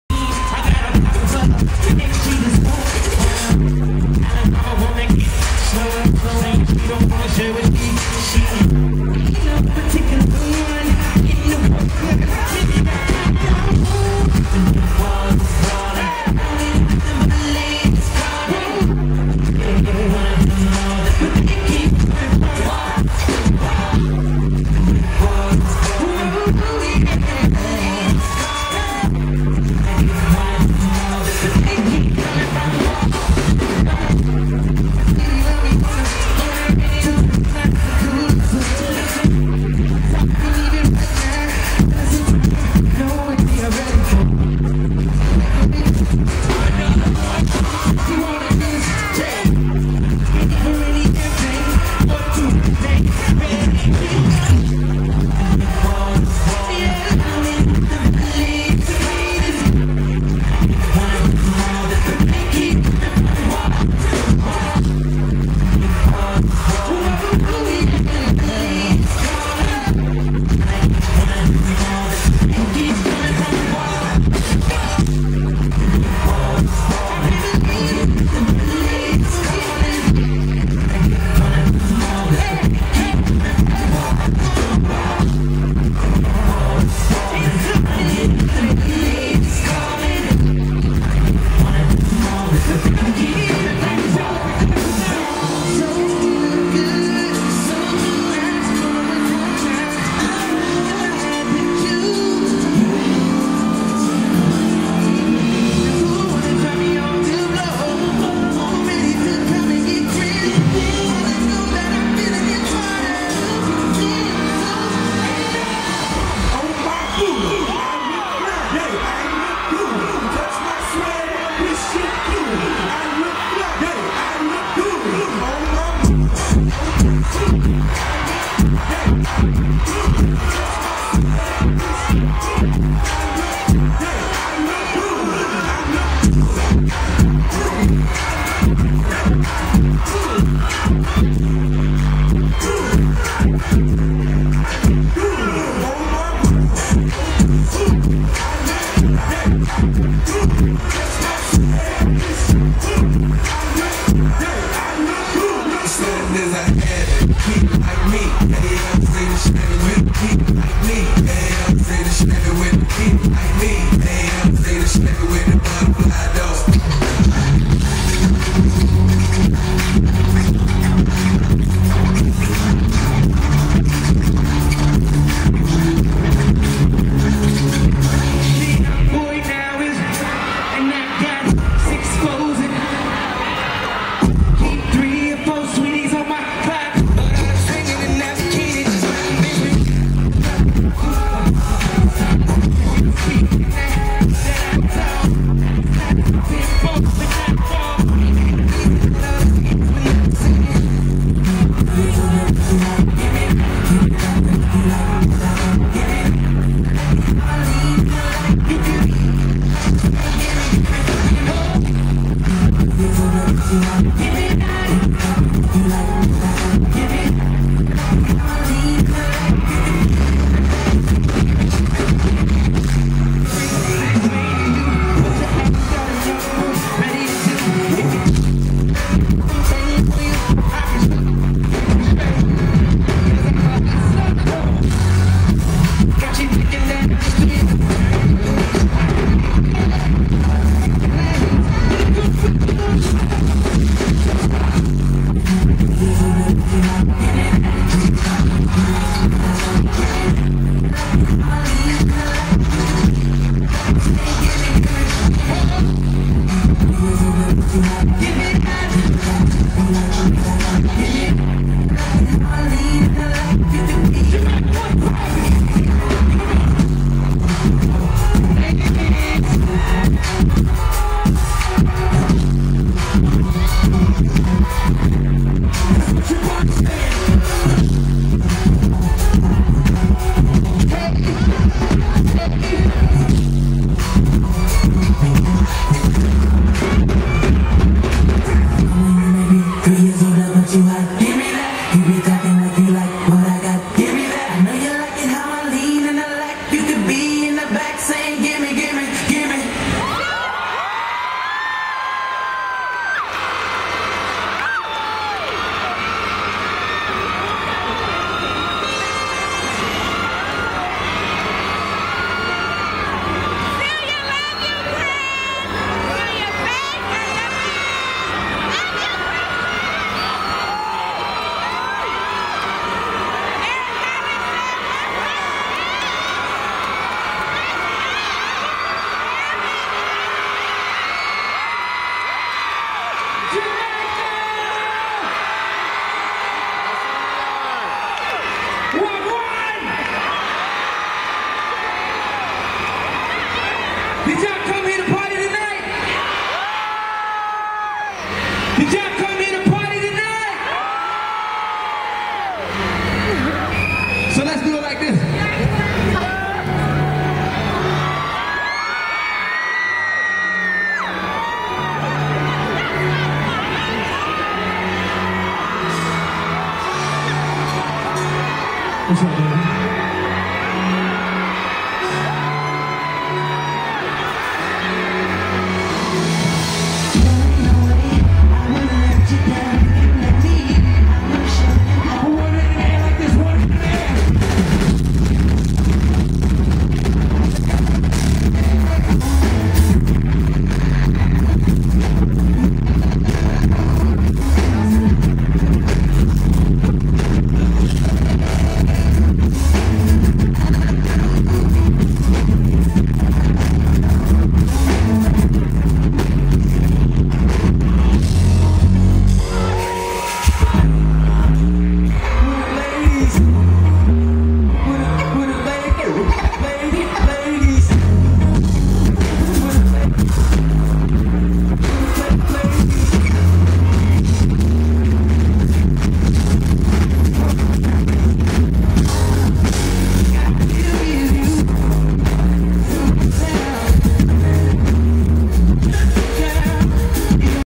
Fan Video of Chris Brown at Reggae Sumfest 2010
Fan Video of Chris Brown at Reggae Sumfest 2010 in July 2010, Montego Bay, Jamaica